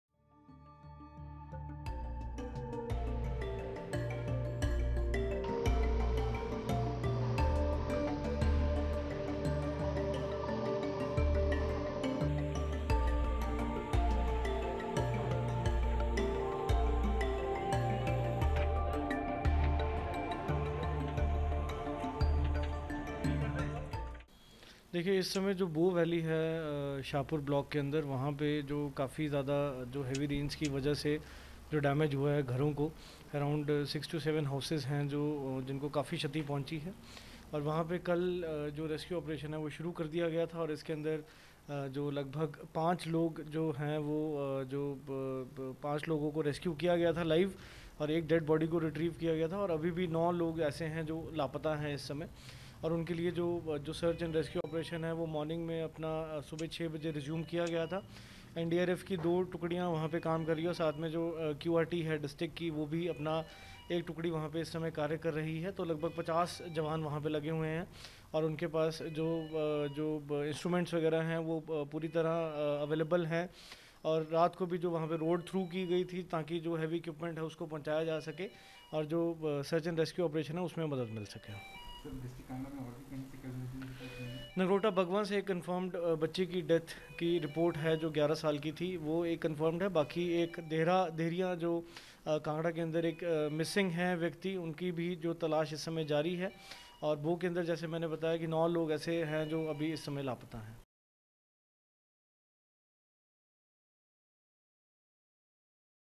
9 missing, several houses damaged in Kangra’s Boh valley after heavy rains
Kangra (Himachal Pradesh), July 13 (ANI): Rescue operations are still underway in Boh valley in the Kangra district of Himachal Pradesh after heavy rains triggered landslides and floods. Nipun Jindal, Deputy Commissioner of Kangra, said, “6-7 houses damaged in Boh Valley. 9 people are still missing. Search and rescue operation by NDRF is underway”. (SA)